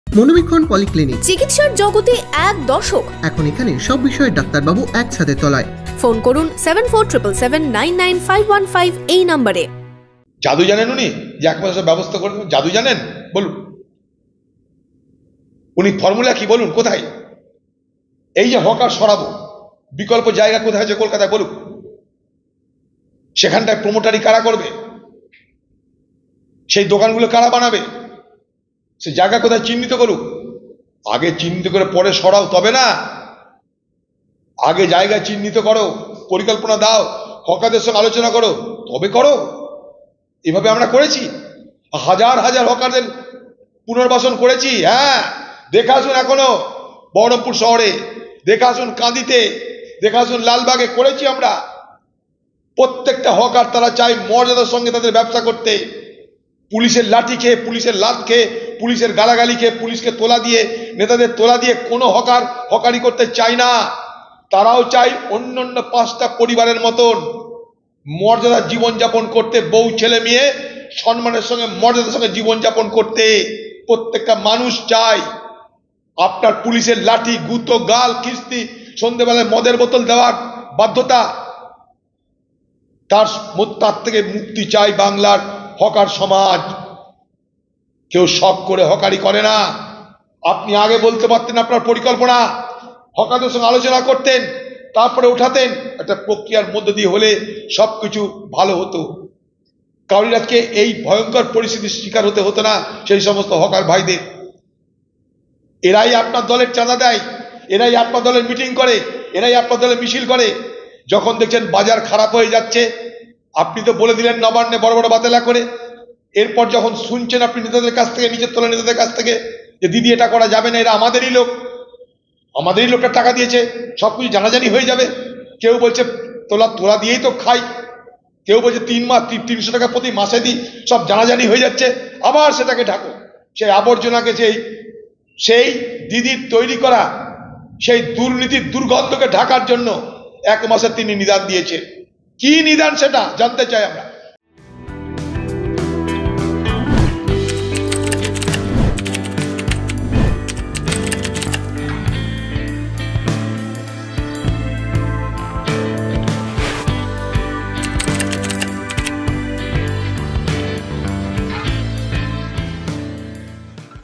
Adhir Chowdhury: হকার উচ্ছেদ নিয়ে মমতাকে তোপ অধীরের
Again Adhir Ranjan Chowdhury attacks Mamata Banerjee. On the topic of Hawkers.